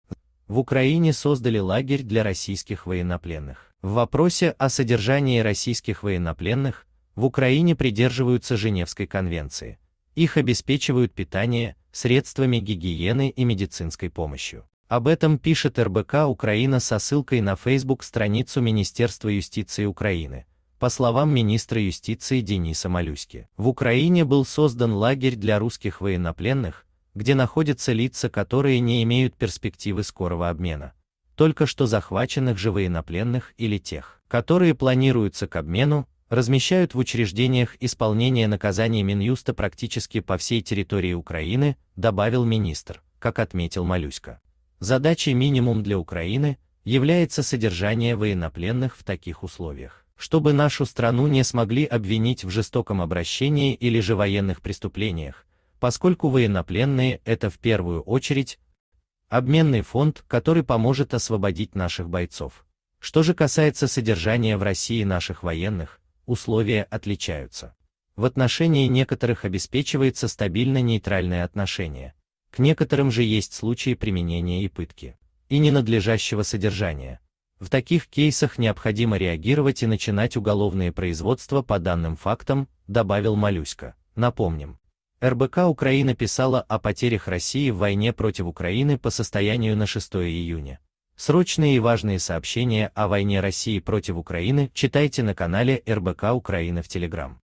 В Украине создали лагерь для российских военнопленных.
РоссийскаяФедерация Украина В вопросе о содержании российских военнопленных, в Украине придерживаются Женевской конвенции. Их обеспечивают питание, средствами гигиены и медицинской помощью. Об этом пишет РБК-Украина со ссылкой на Facebook-страницу Министерство юстиции Украины. По словам министра юстиции Дениса Малюськи, в Украине был создан лагерь для русских военнопленных, где находятся лица, которые не имеют перспективы скорого обмена. "Только что захваченных же военнопленных или тех, которые планируются к обмену, размещают